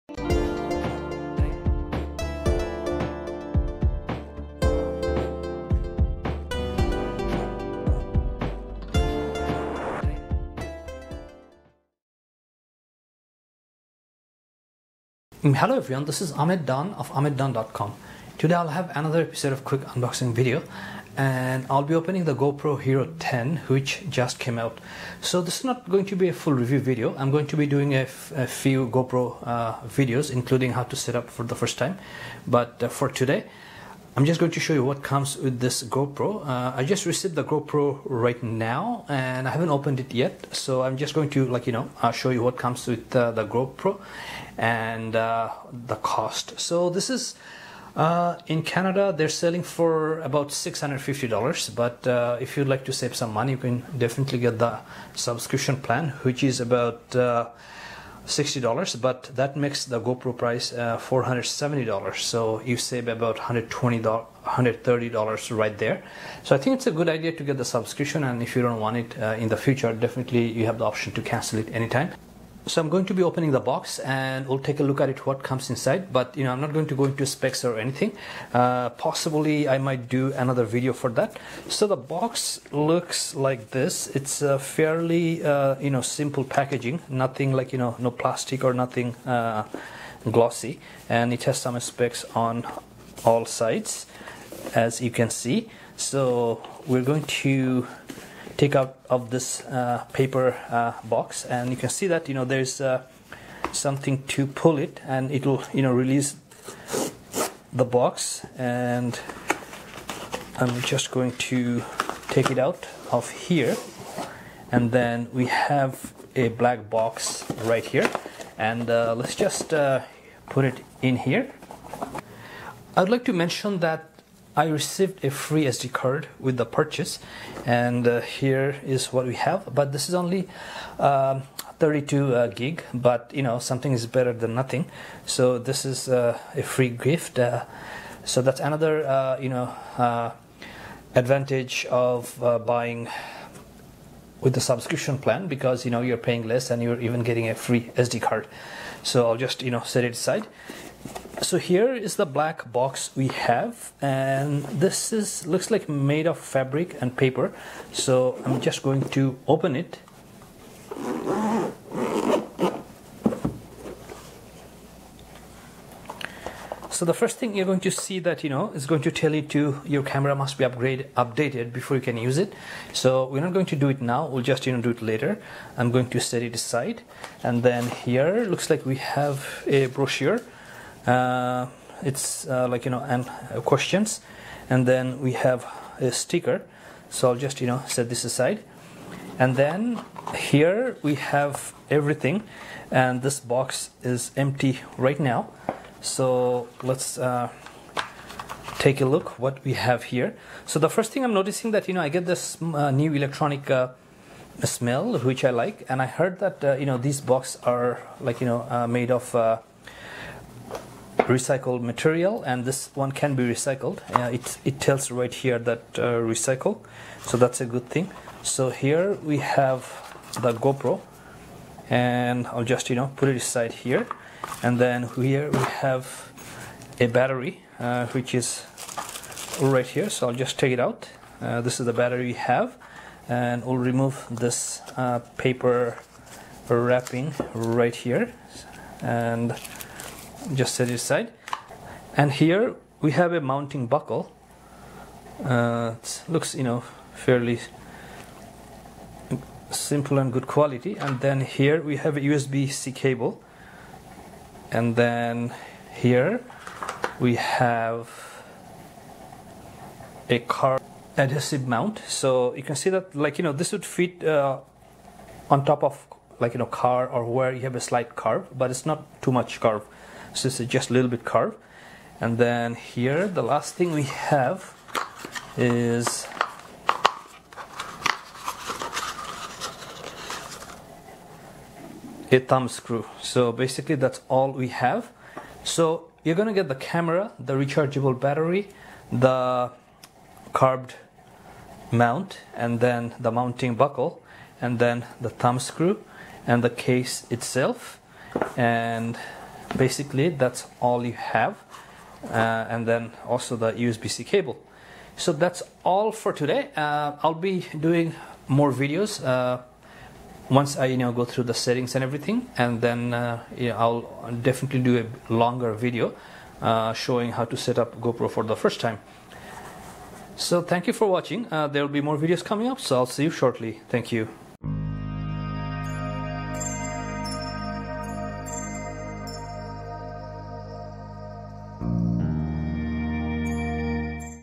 How I Saved $120 on GoPro 10 & What Comes With the New GoPro 10
GoPro 10 Fast Unboxing & What Else You Get in the Box | GoPro Hero 10 Black Unboxing

My last GoPro was Hero 7. I was waiting for GoPro 10 to come out. This year, I will make some tutorial videos on GoPro 10, instead of one long unboxing and review video.

In this video, I will talk about how to save money buying GoPro and we will unbox Hero 10 which just arrived today.

Chapters:
0:00 Intro
0:16 GoPro Hero 10 Has Arrived
0:45 How to Save Money on GoPro 10
1:45 GoPro 10 Unboxing
2:05 Free SD Card with GoPro 10
2:45 What's Inside GoPro Hero 10 Black Box
4:45 Summary of What Comes with GoPro 10